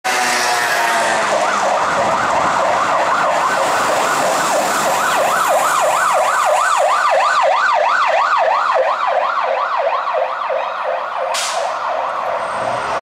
Humberside Police - Vauxhall Vivaro Cell Van On Shout
One Of Humberside Police's Newer Cell Vans On A Shout Through Hull. Watch In 1080p.